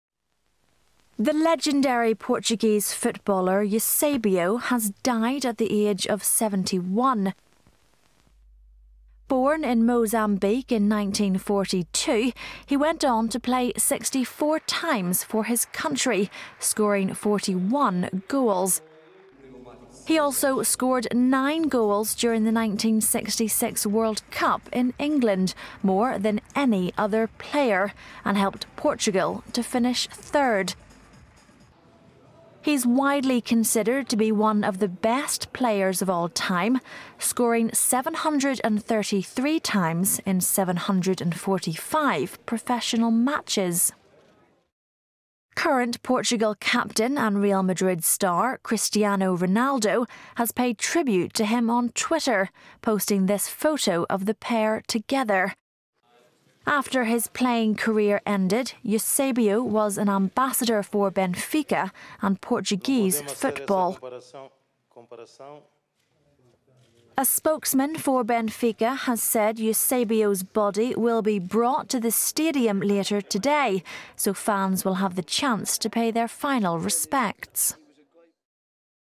Portuguese football legend Eusebio has died aged 71
The legendary Portuguese footballer Eusebio has died at the age of 71. Born in Mozambique in 1942, he went on to play 64 times for his country, scoring 41 goals. He also scored nine goals during the 1966 World Cup in England, more than any other player, and helped Portugal to finish third. He is widely considered to be one of the best players of all-time, scoring 733 times in 745 professional matches. After his playing career ended, Eusebio was an ambassador for Benfica and Portuguese football. A spokesman for Benfica has said Eusebio's body will be brought to the stadium on Sunday so fans will have the chance to pay their final respects. Report by Sarah Johnston.

The strangest news trend of 2013? Kids stuck in China
Incredible! Elephants play football in Nepal